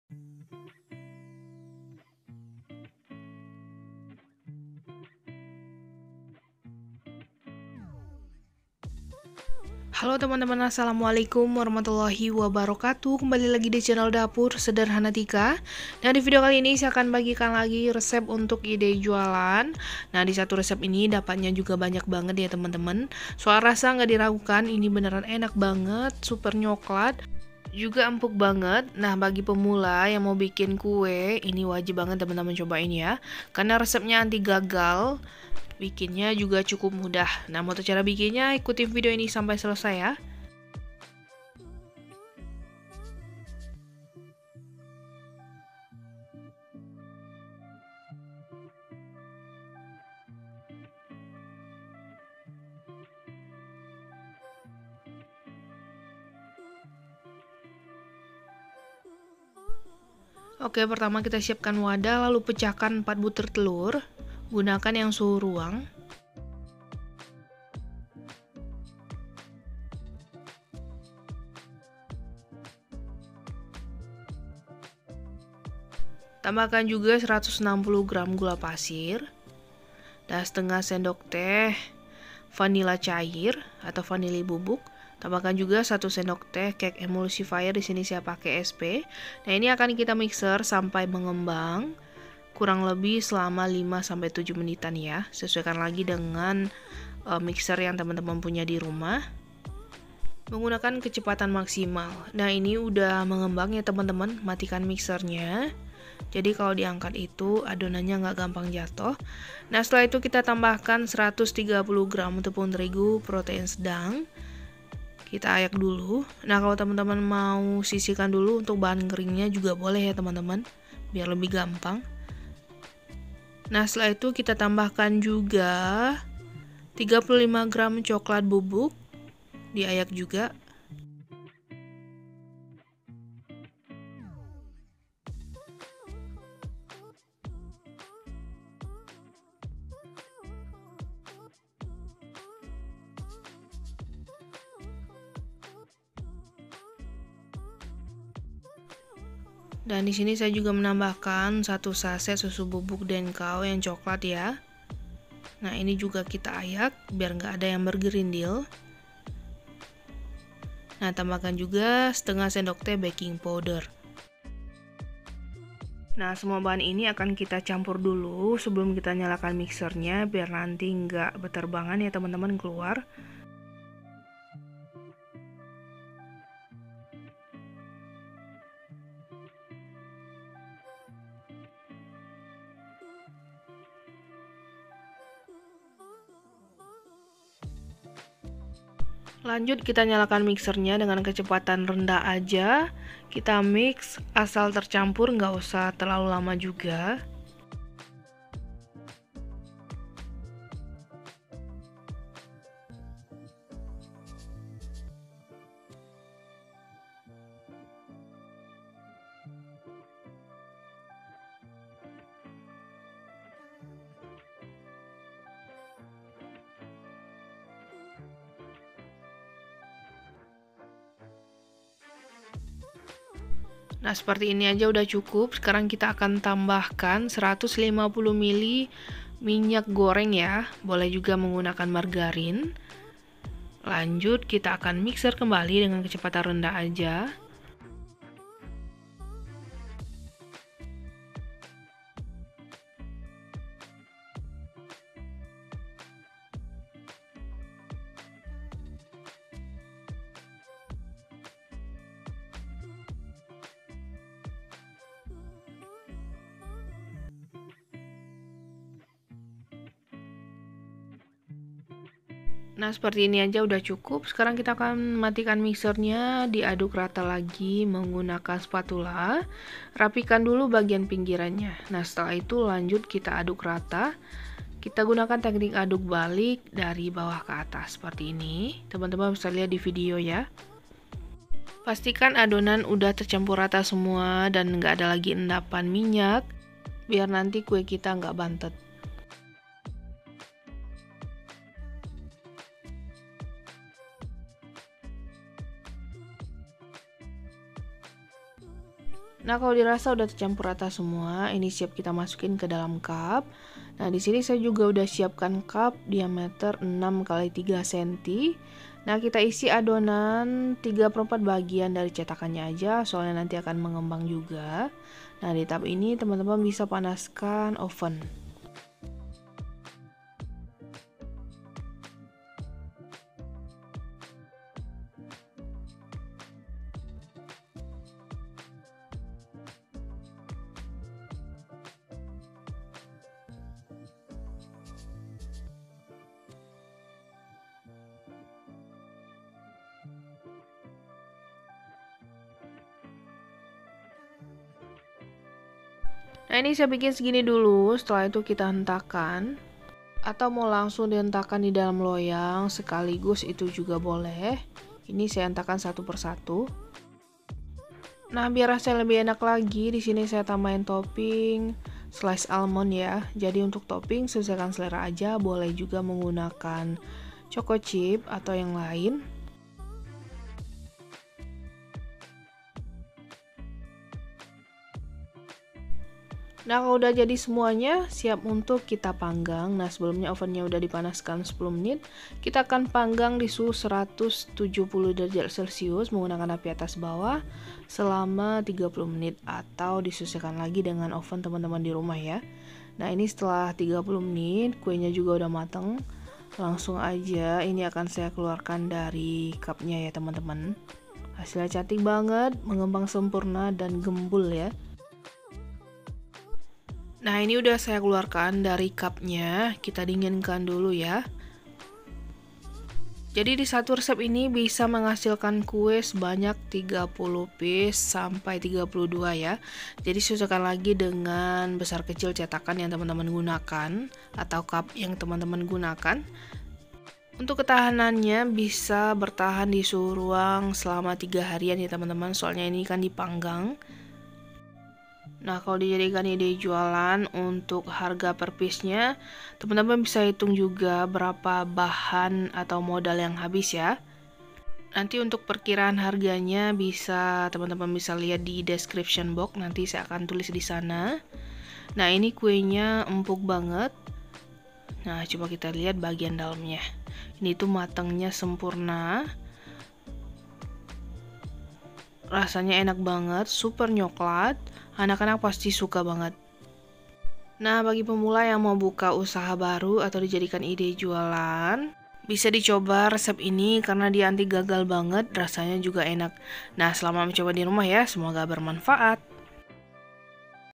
BARU COBA PERDANA TERNYATA ENAK BANGET PAS UNTUK IDE JUALAN 1 RESEP DAPATNYA JUGA BANYAK ANTI GAGAL
🎀 BOLU COKLAT 🎀

  Bahan :

🌿 4 butir telur  : 7.600
🌿 160 gr gula pasir : 2.160
🌿 1 sdt sp : 1.000
🌿 1/2 sdt vanila cair : 250
🌿 130 gr tepung terigu pro sedang : 2.210
🌿 35 gr coklat bubuk : 7.000
🌿 1 sachet susu bubuk coklat : 4.000
🌿 1/2 sdt baking powder : 250
🌿 150 ml Minyak goreng : 2.700

Total Kue : 30-32 pcs
Diameter cetakan 6× 3 cm

Perkiraan harga jual : 1.500 × 32 : 48.000
                                      2.000 × 32 : 64.000

Blom termasuk cup & listrik ya
Tinggal disesuaikan lagi aja ya guys !!

Selamat mencoba...